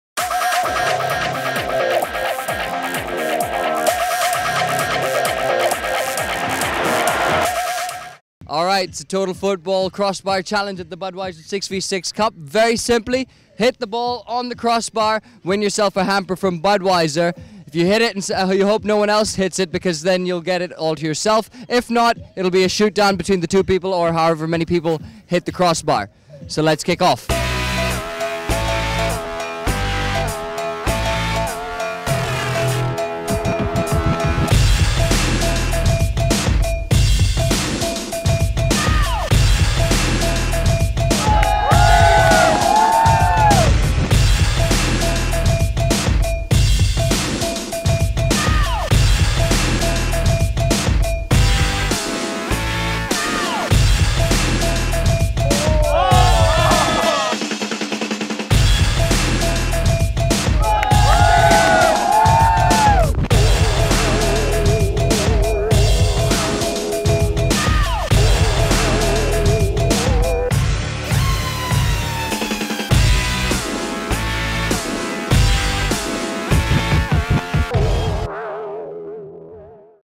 Total Football : Crossbar Challenge
Total Football brings you the crossbar challenge from Budweiser 6v6 Cup, Mumbai. Good feet and accuracy is hard to master, with three players tied in the end but only one to walk away with the Budweiser hamper. Enjoy!!

Keep watching total football for all the action from the Budweiser 6v6 cup in the coming month.